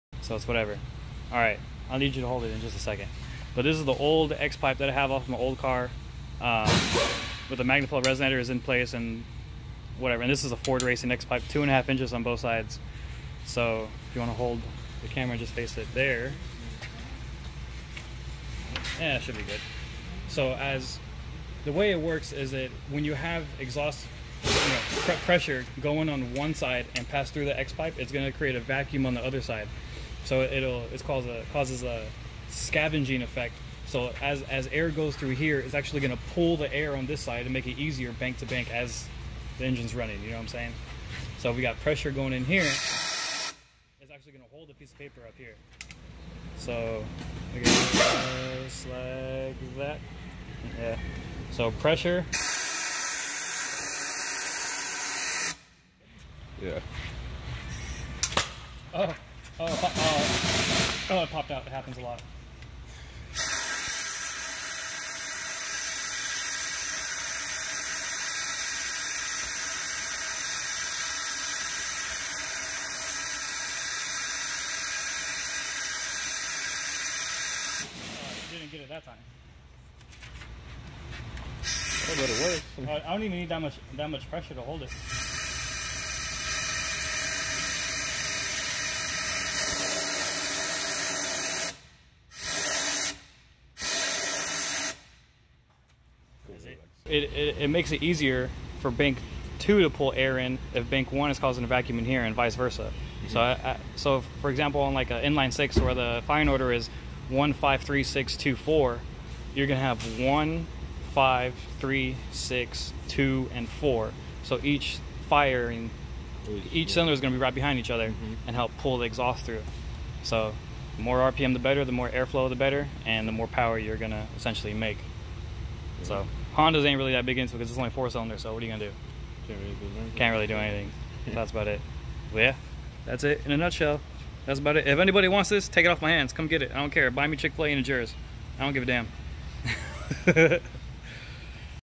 Watch how a X pipe works! | How things work
Watch how a X pipe works! | How things work | Y Legal Racing

In today's video we take a look at how a X pipe works. This video will be handy for anyone that is curious to how it works or who has never seen the full scavenge effect in action!